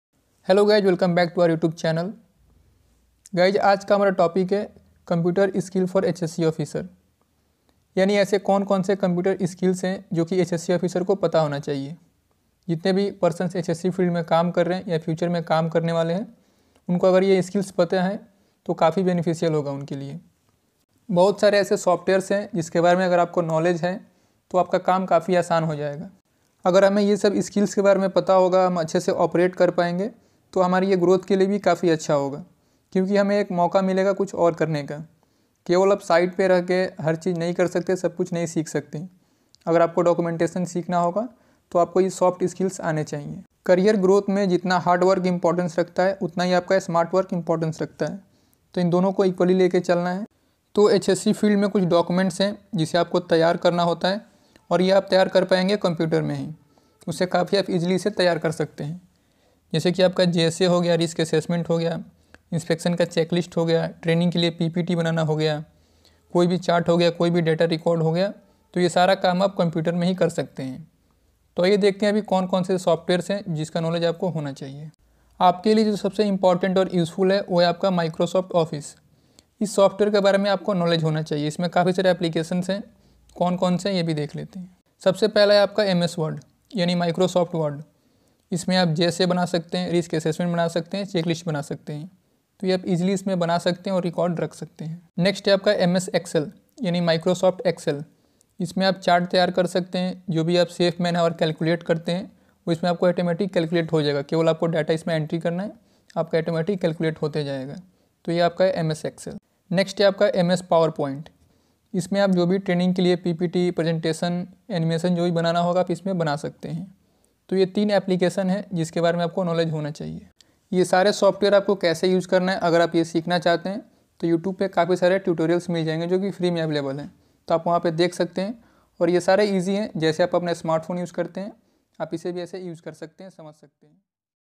Important Computer Skills for HSE Professionals / Safety Officer
If You Want to Succeed in Your Hse Professional Then learn these important computer skills.

safetymgmtstudy